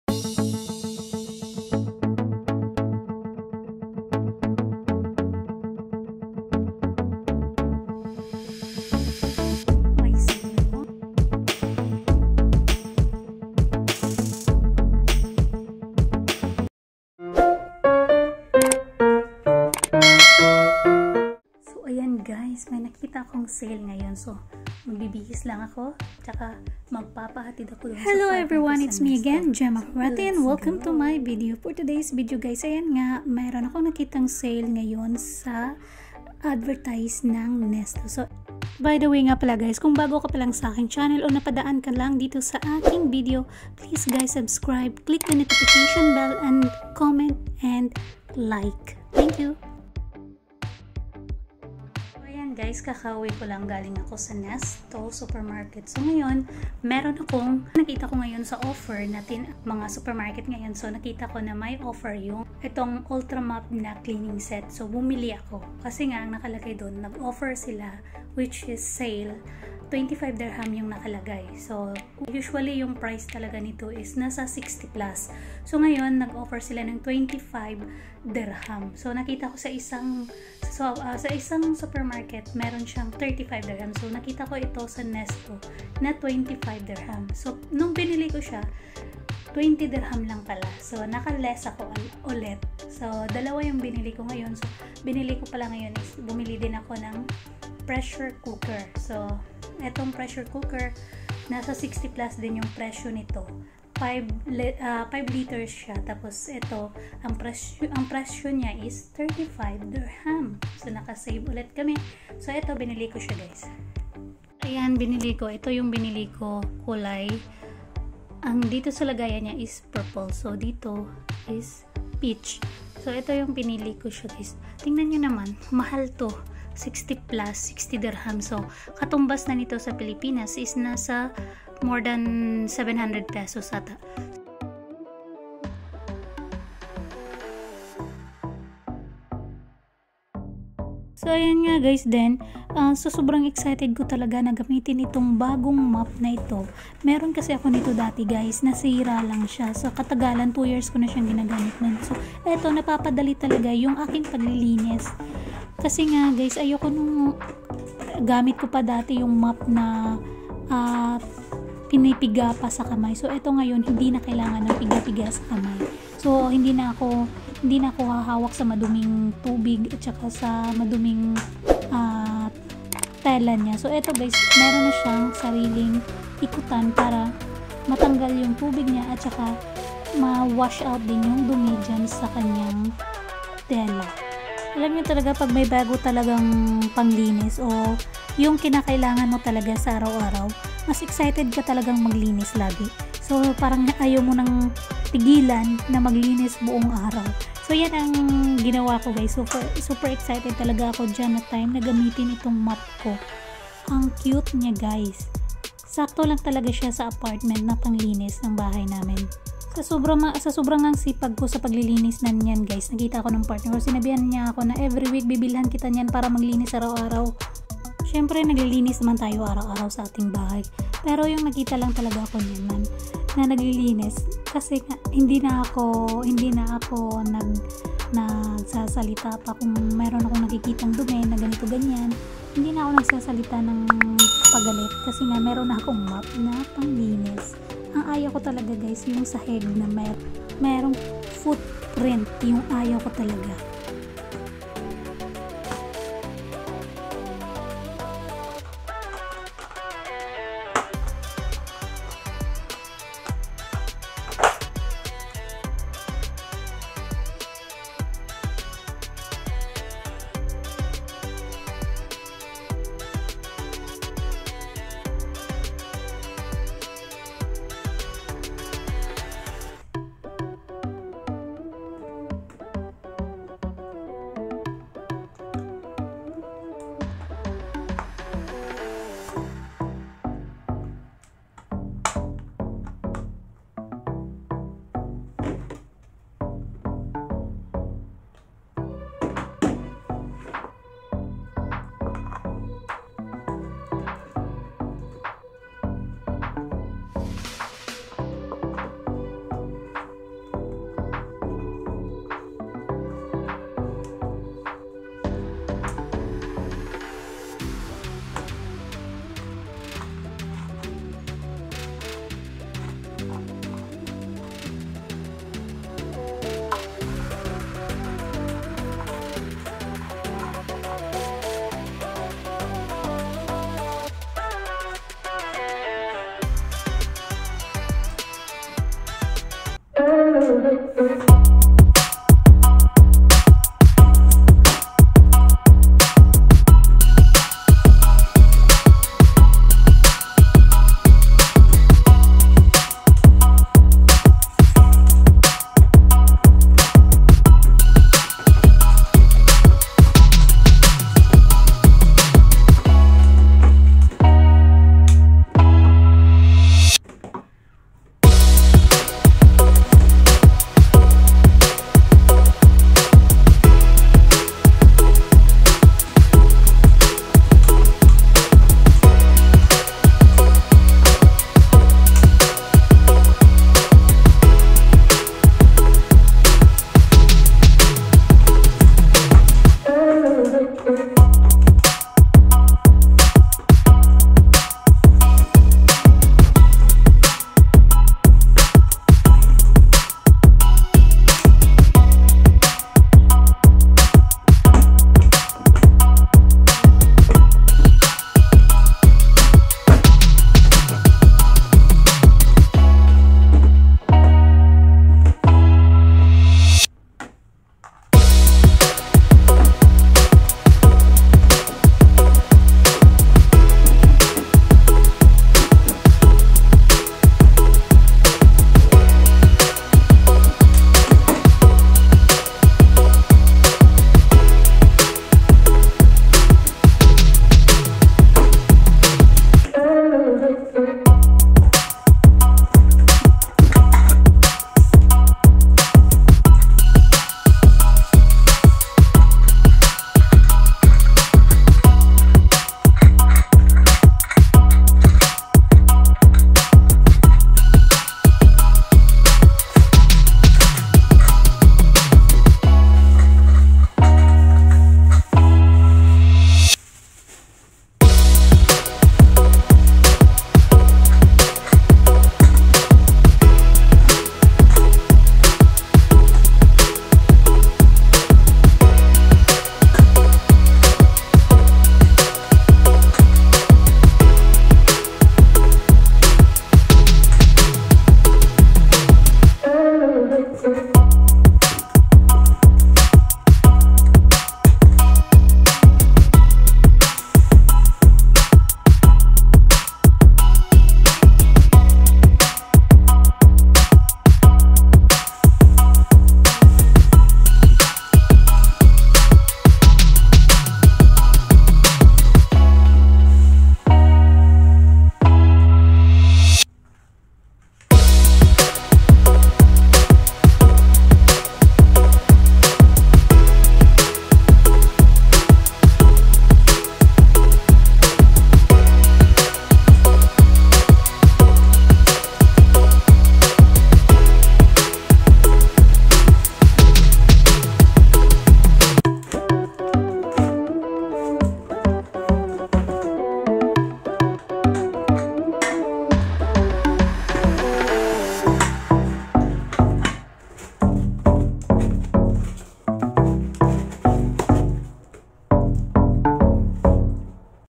VLOG IN Dubai ||GRABE YUNG SALE HALOS KALAHATI NG PRESYO//Gemma Karate buying saleultramap#
I buy rotating mop with super sale more than half of the original price and good quality for home cleaning,and i also buy pressure cooker and same half of the original price lower.